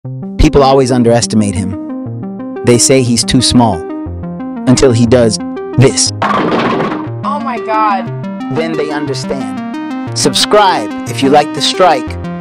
Big Strikes Come In Small Packages
People Would always underestimate him because of his small size. He quiets the doubter with one strike.